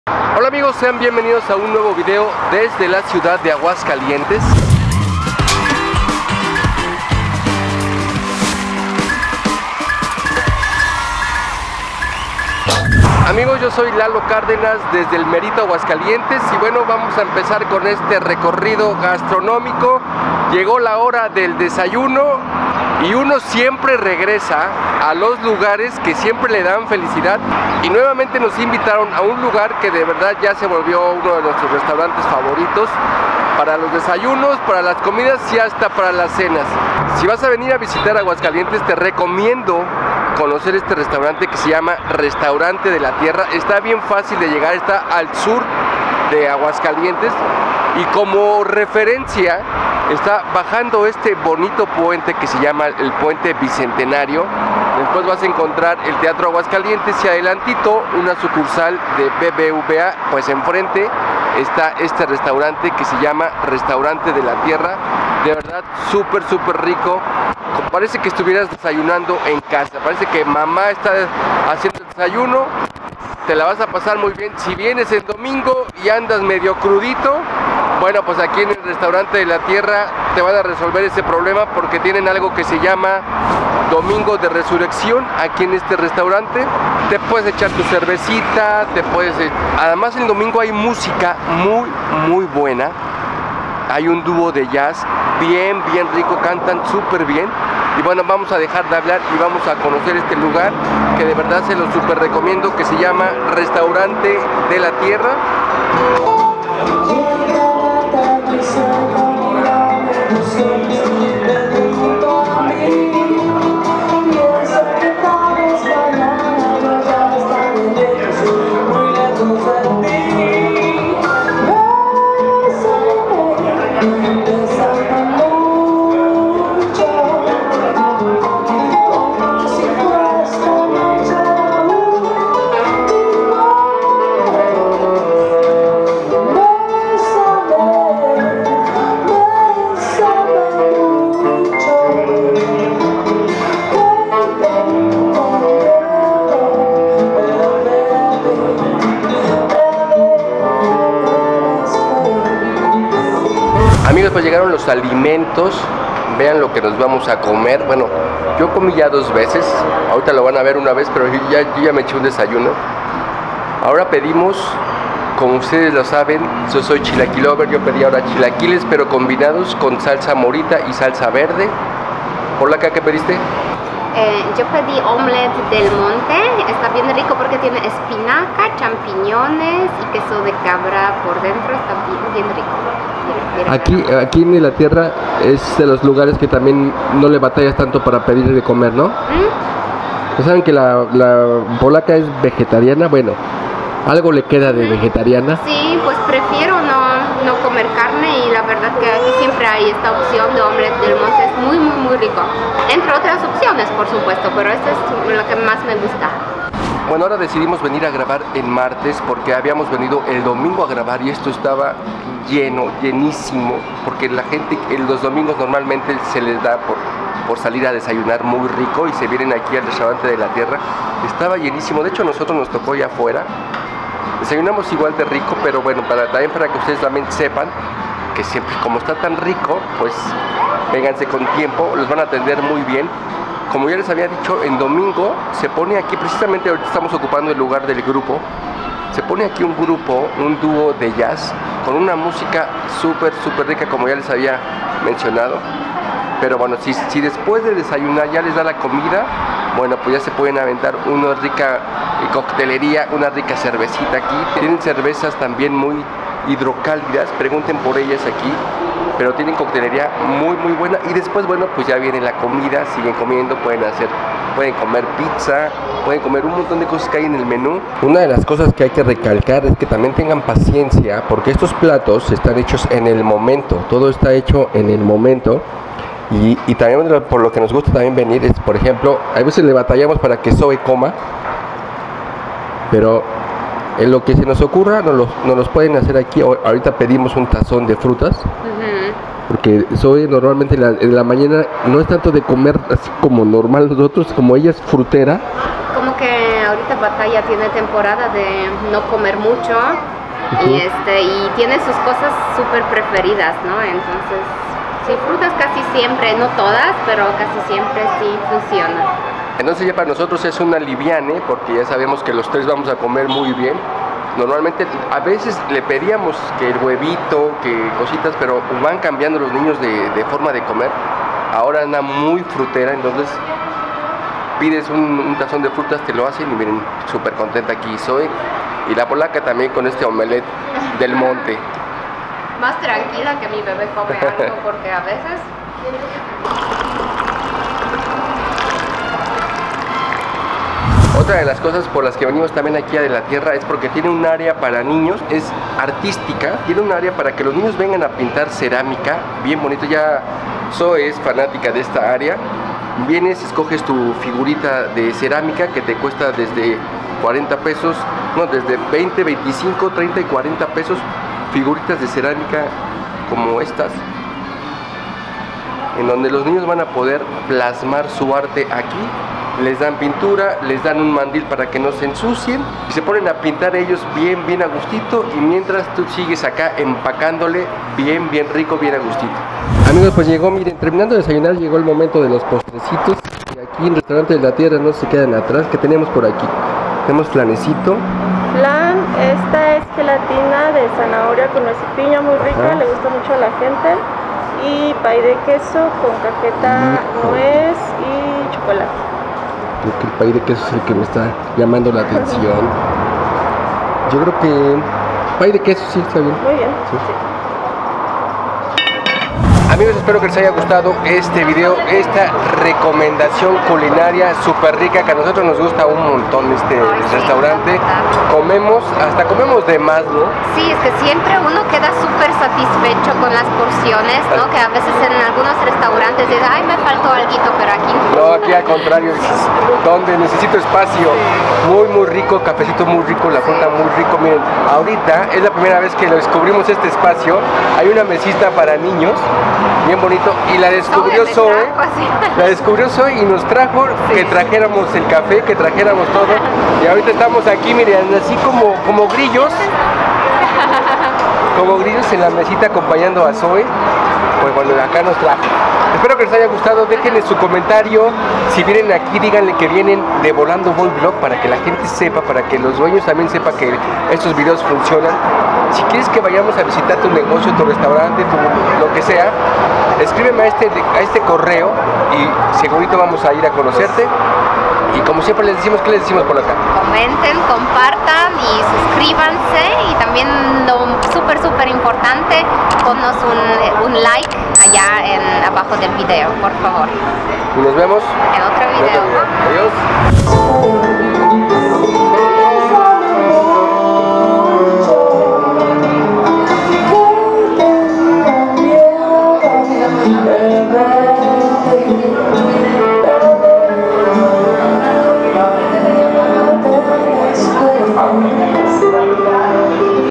PROBAMOS los MÁS RICOS DESAYUNOS de la COCINA TRADICIONAL en AGUASCALIENTES
Restaurante de la Tierra.
Av. Aguascalientes Sur num 2713
Jardines de las fuentes, Ags, MX
Pedidos: .449363 49 80

Te dejo unos video que te pueden gustar